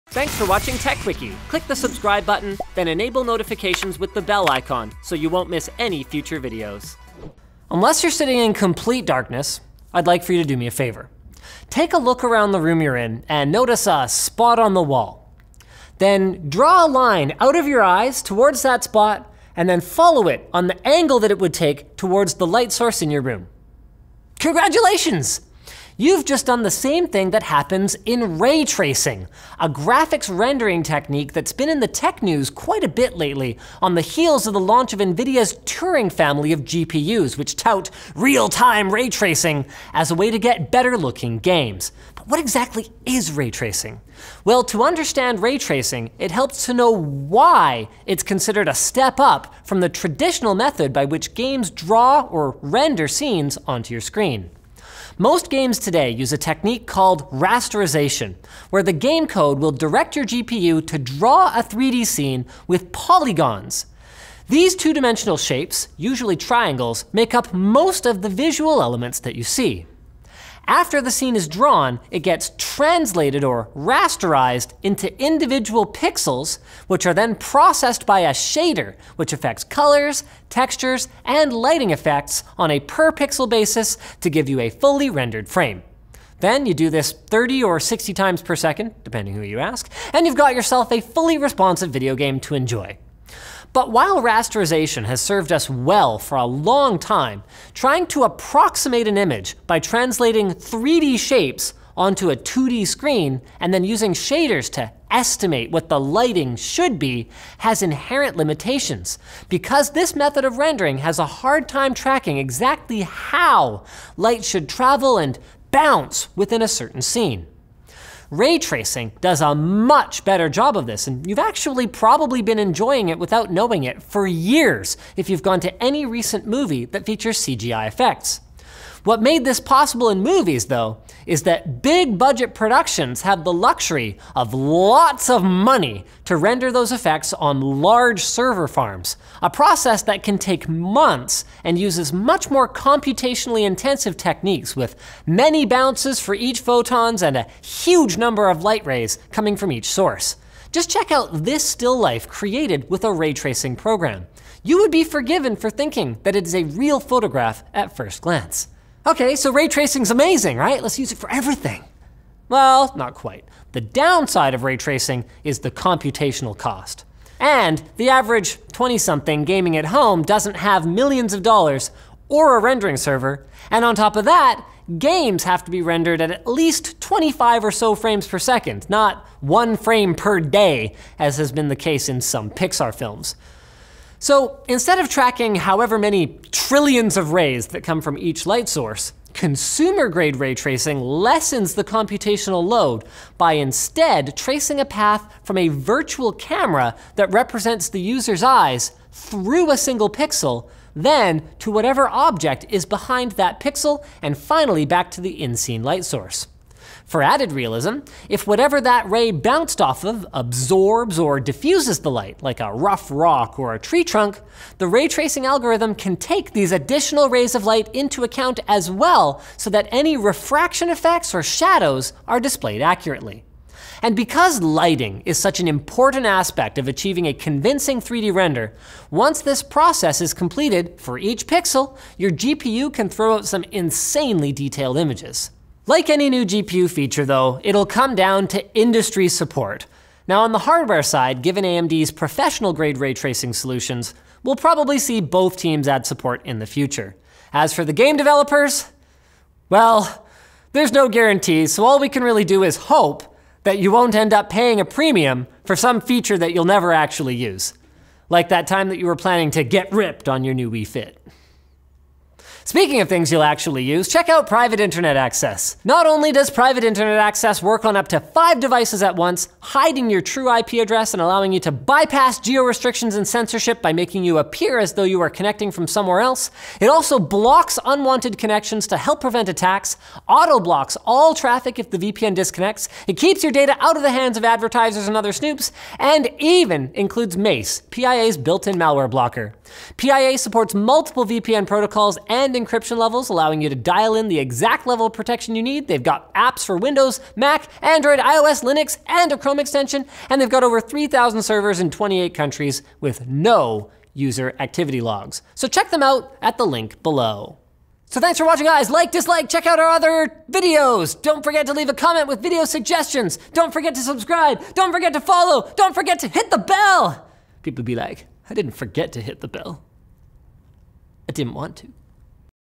What is Ray Tracing?
Ray tracing has made headlines lately as the rendering method of the future for games - but what exactly is it, and how can it make your games look more realistic?